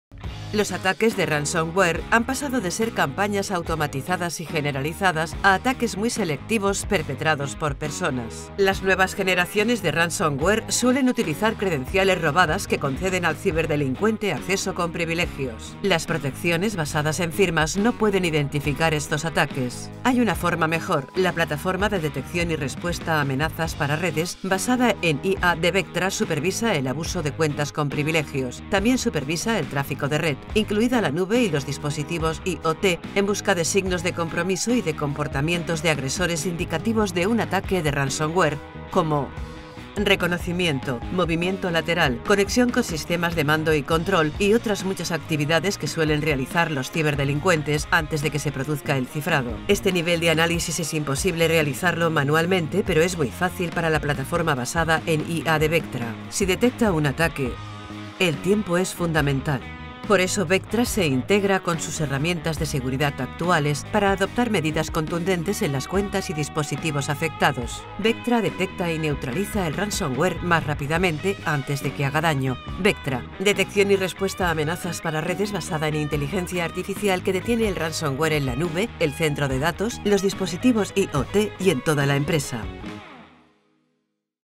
Vectra detiene el ransomware antes del cifrado
Aprenda a detectar y detener el ransomware para evitar el cifrado de sus datos corporativos. Con la detección y respuesta a la red impulsada por la IA de Vectra Cognito, puede ver cómo el ransomware se mueve por la red, cómo se mueve lateralmente, cómo evade las defensas y cómo se mantiene activo.  Sólo Vectra Cognito puede rastrear y vincular estas actividades desde la red al centro de datos y a la nube. Para saber más sobre cómo detener el ransomware, visite

Vectra es la Seguridad que piensa.